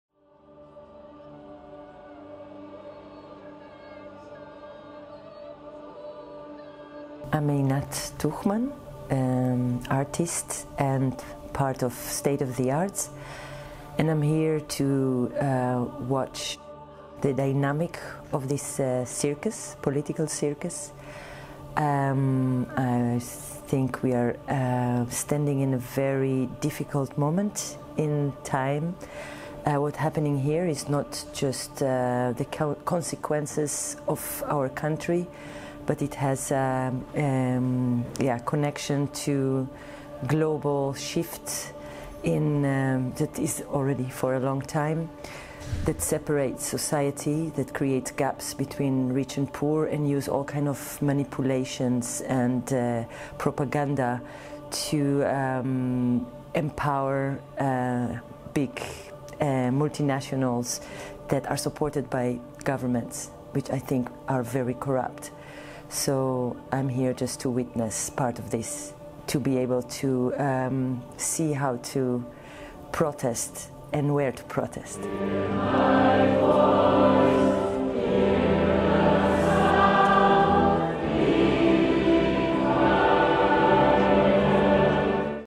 Einat Tuchman over de besparingen
Begrotingsdebat Vlaams parlement, 19 december 2019